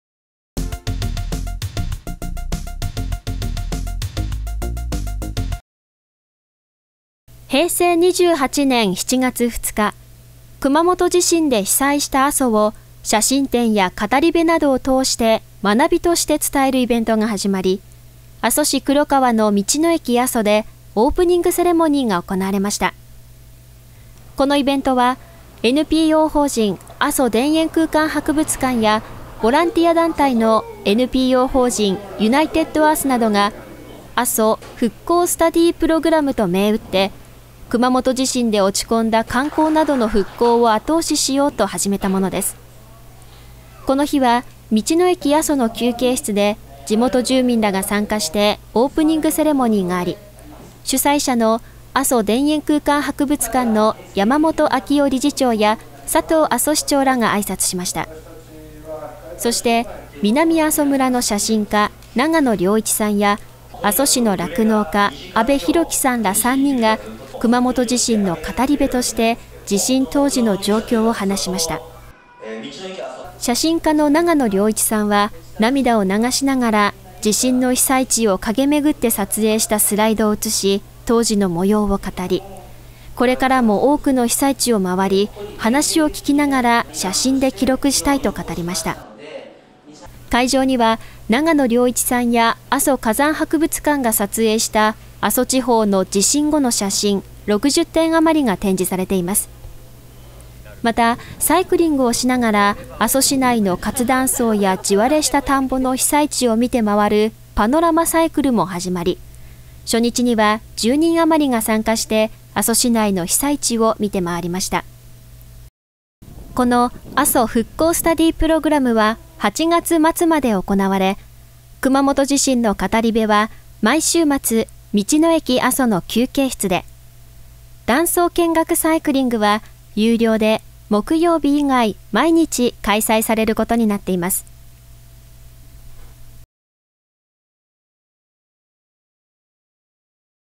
ASO復光スタディープログラムオープニングセレモニー
道の駅阿蘇でASO復興スタディープログラムのオープニングセレモニーが開かれました。